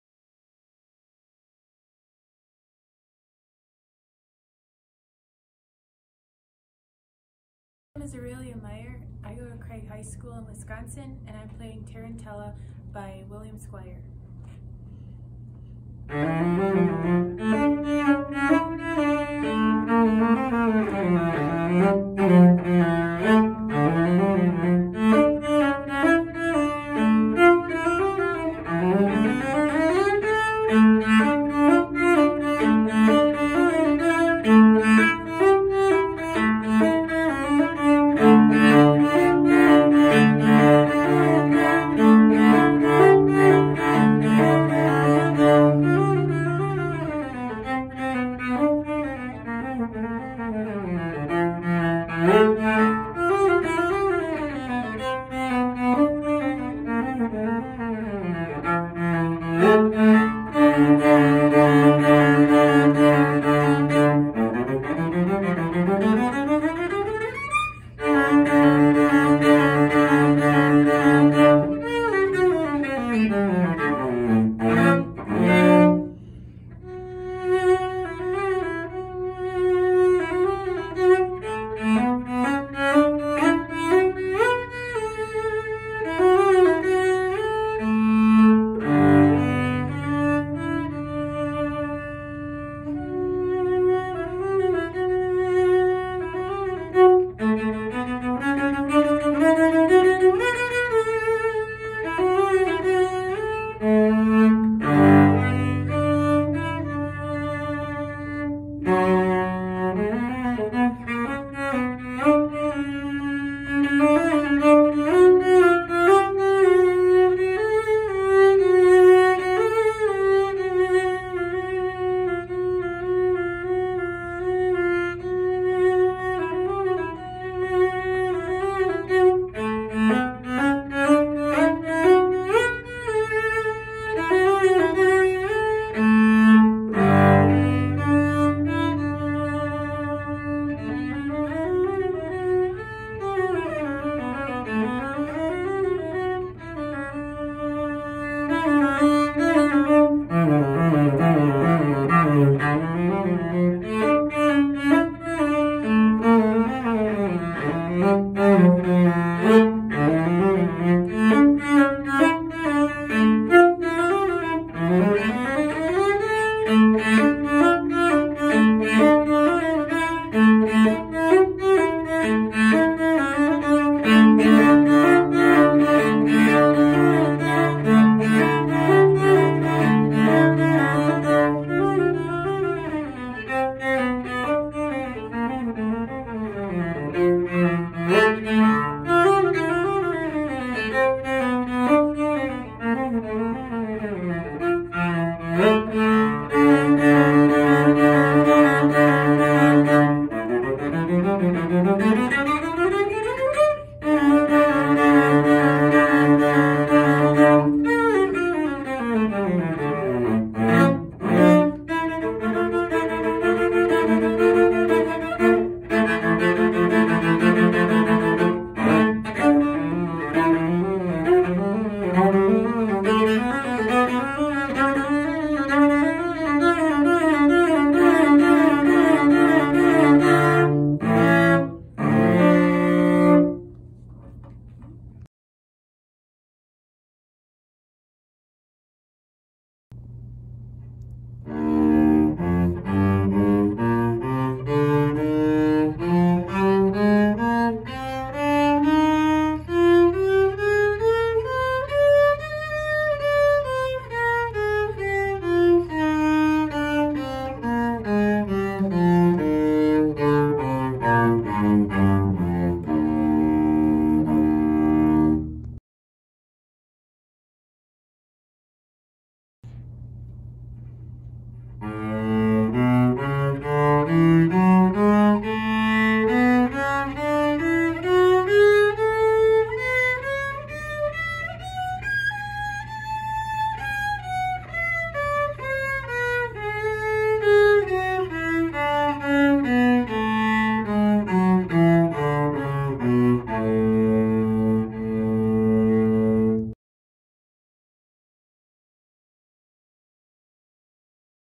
Aurelia Meyer Audition Video for MYSO 2022
Aurelia Meyer from Craig H.S. in Janesville audition video for MYSO